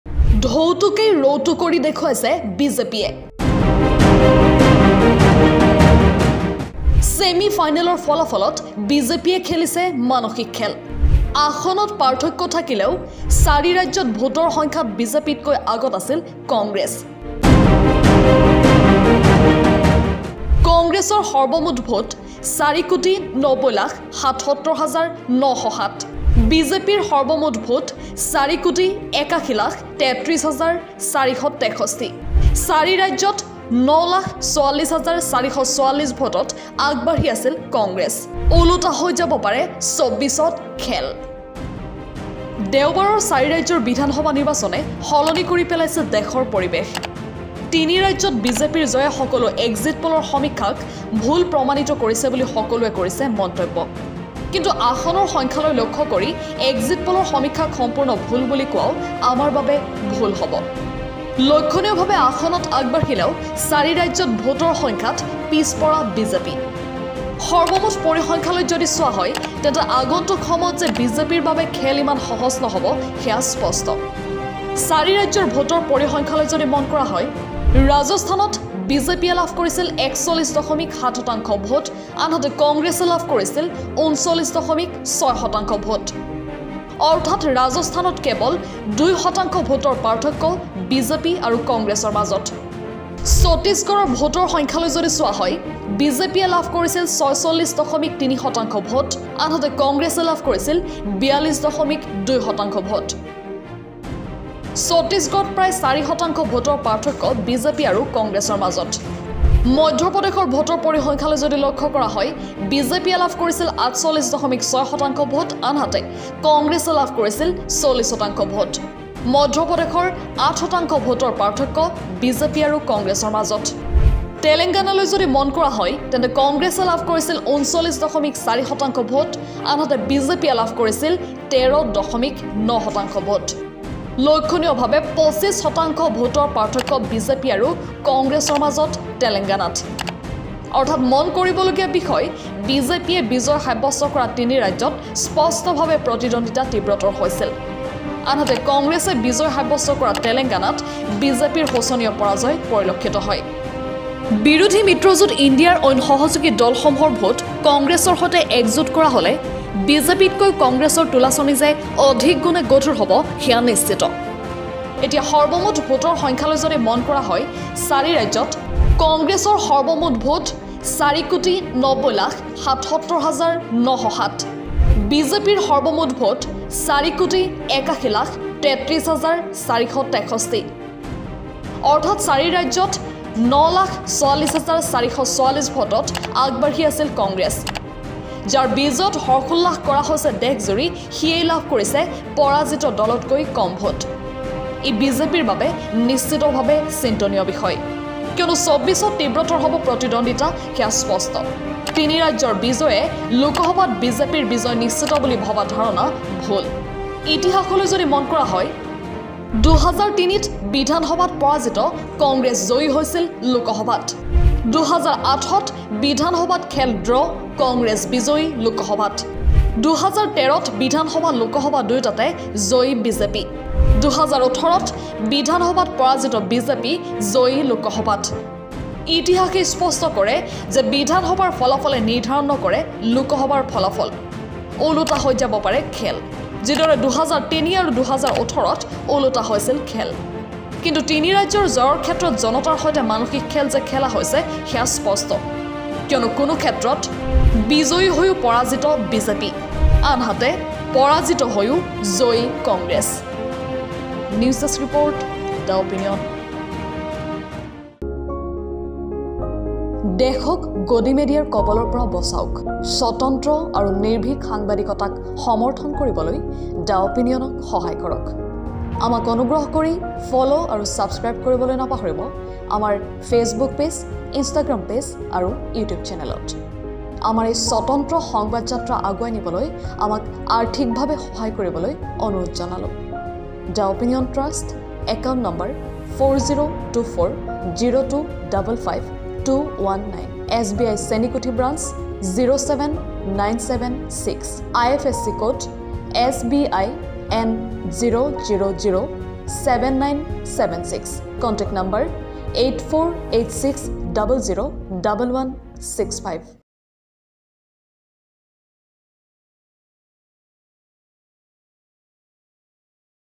চাৰি ৰাজ্যত ভোটৰ সংখ্যাত বিজেপিতকৈ আগত কংগ্ৰেছ
ঢৌটোকেই ৰৌতো কৰি দেখুৱাইছে বিজেপিয়ে
📌৯,৪৪,৪৪৪ ভোটত বিজেপিতকৈ আগত কংগ্ৰেছ